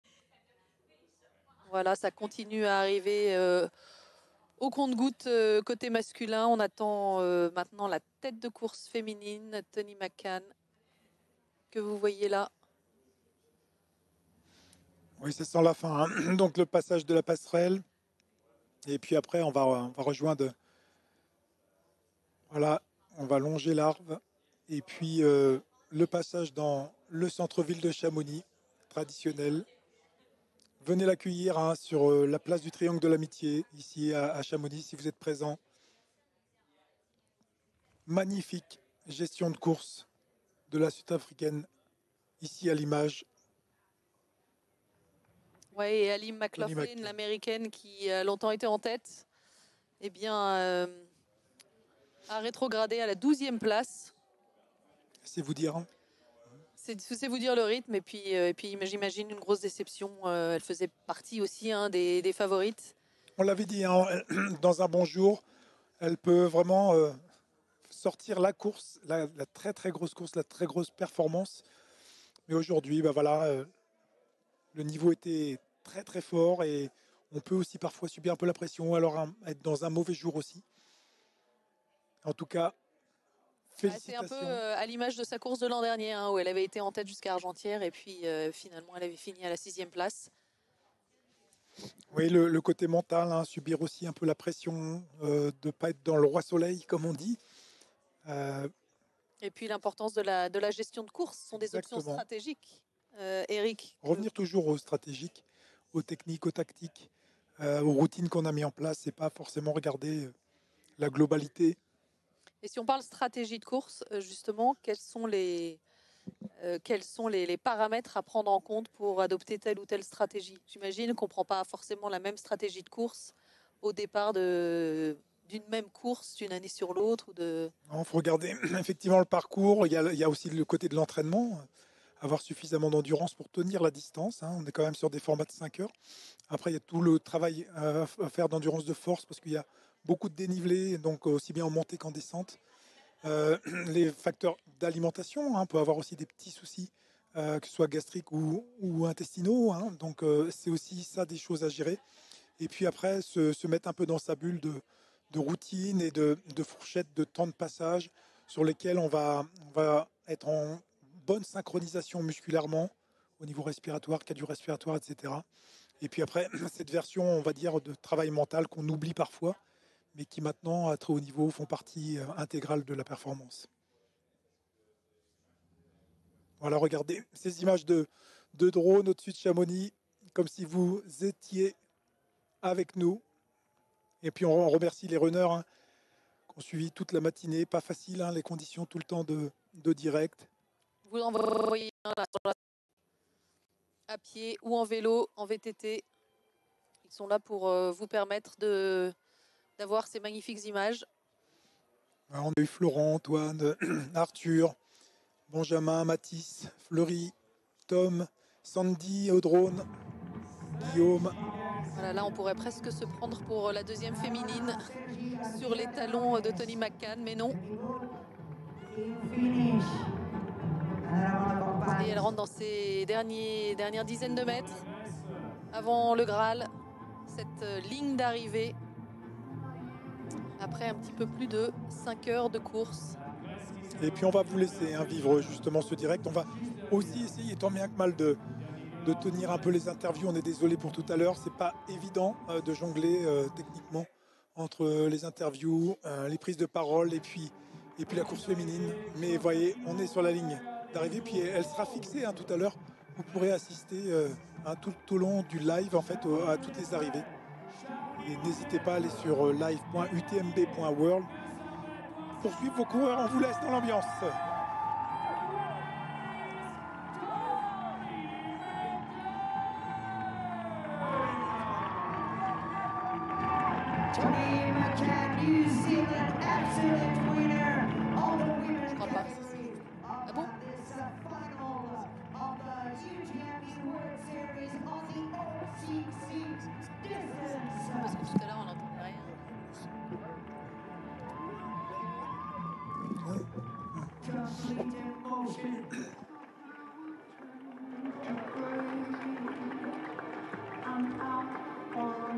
Ultratrail Mont-Blanc - La surprenante Toni McCann scotche toutes la favorites
Chez les femmes, c'est la surprenante Sud-Africaine Toni McCann qui s'impose en 5h18' devant les favorites Katie Schide (vainqueure de l'UTMB 2022) et Mia Yao (2e du Marathon du Mont-Blanc 2023). Elle devance l'Américaine Schide de près de 8 minutes, cette dernière ayant terminé au coude à coude avec la Chinoise Yao (elles arrivent dans la même minute sur la ligne d'arrivée chamoniarde). La première Française, Candice Fertin, prend la 7e place.